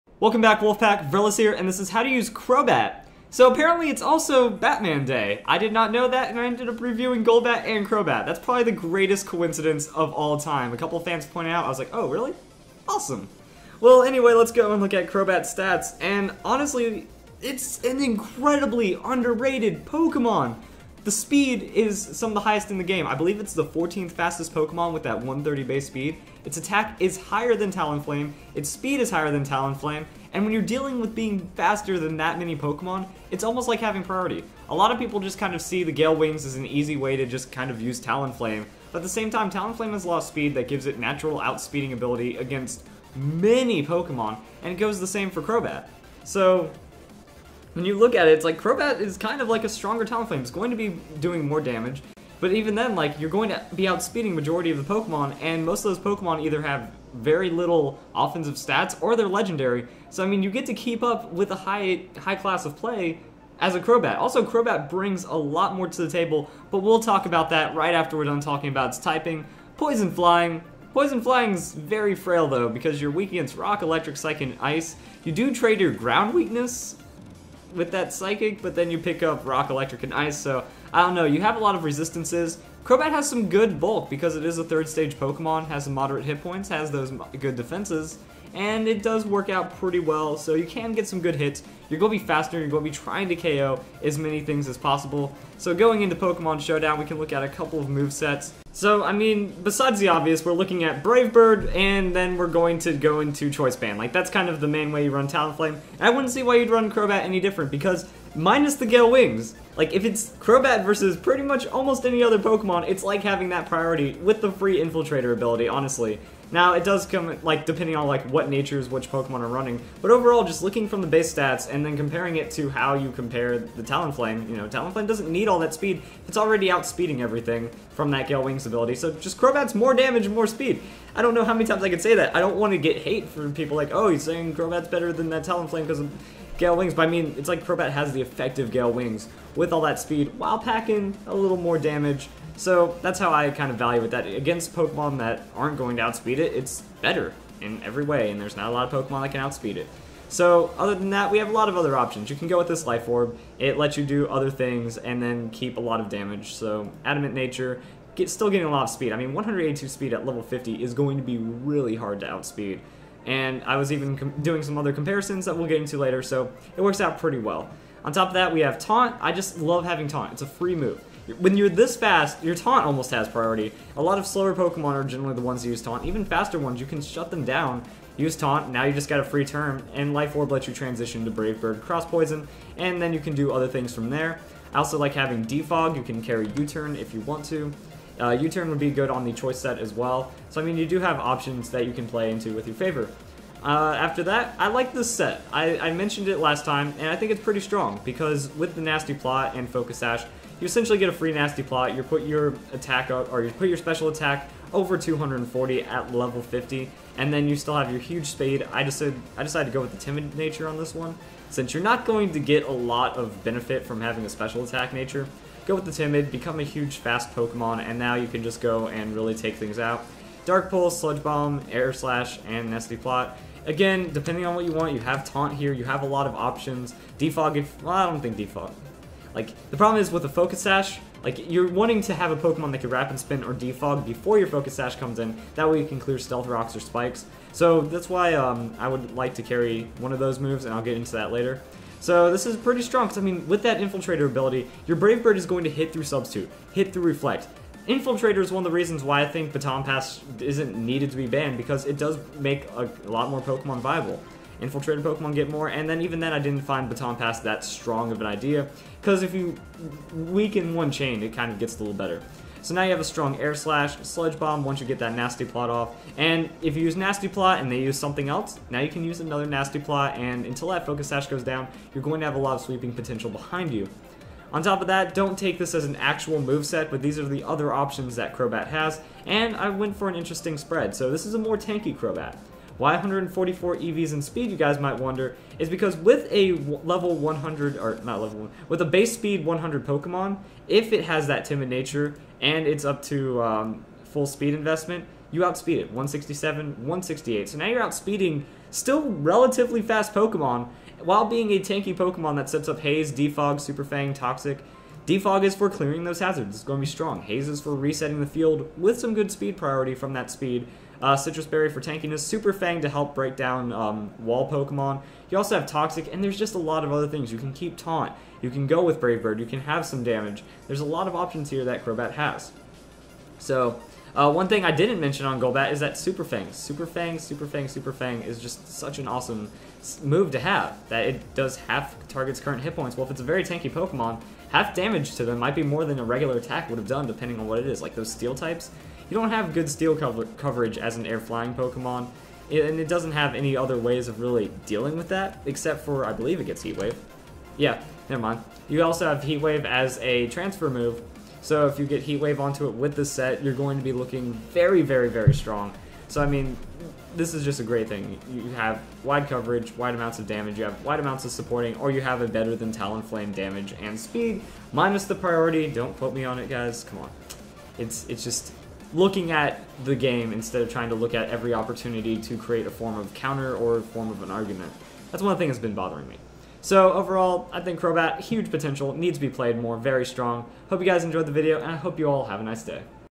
How To Use: Crobat! Crobat Strategy Guide!
I have already reviewed Crobat before, but it is an unbelievably underrated Pokemon. More Base Speed and Attack than Talonflame while still packing more Hitpoints. Even comparing it to gale wings, Crobat's speed makes it the 14th fastest Pokemon (Including Legendaries) so its like having an effective priority against most Pokemon since you outspeed anyway. Crobat also has great support or special potential so it can be very unpredictable. Crobat4Ubers